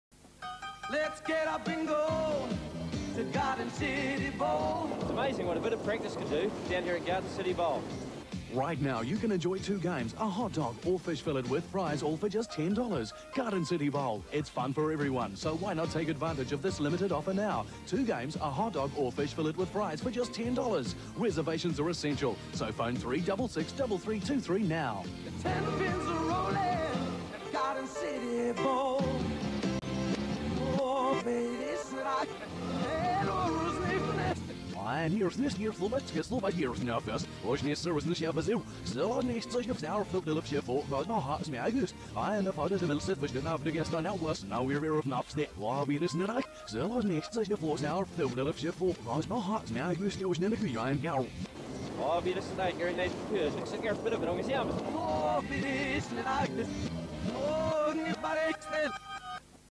Garden City Bowl ad with Rod Latham
Former Black Cap Rod Latham trades one form of bowling for another in this advert for Garden City Bowl on Iversen Tce.  Watch out for a fun little experiment at the end.